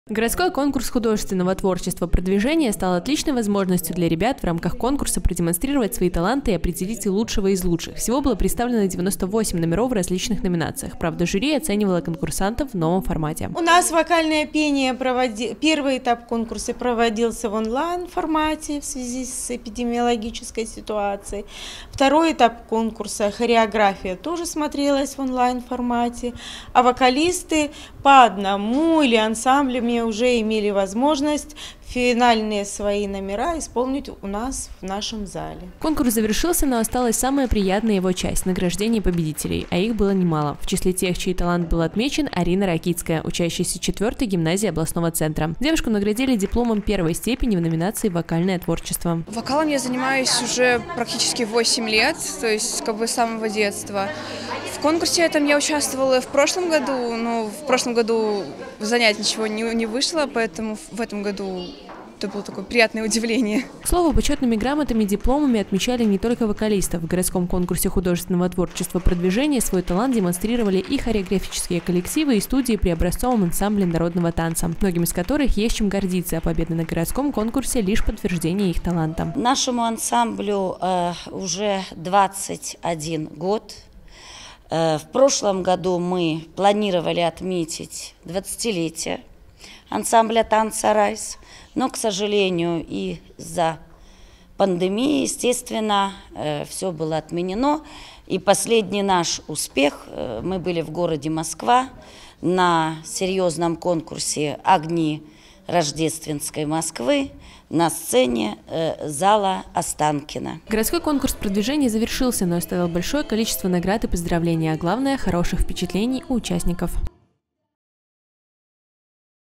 2021-03-26 г. Брест. Итоги городского конкурса «Продвижение». Новости на Буг-ТВ.
Журналист И. Левчук. Оператор А. Щерба
Центр дополнительного образования детей и молодёжи Бреста – оазис творчества и вдохновения. Здесь часто проводят различные конкурсы и доказывают, не словом, а делом, что юные брестчане имеют много творческих способностей. Очередным подтверждением вышесказанного стал городской конкурс художественного творчества «PROДвижение». В нем приняли участие порядка 450 учащихся из 22 учреждений образования областного центра. Всего было представлено 98 номеров в различных номинациях. Подробнее о конкурсе и об его участниках, узнавали наши корреспонденты.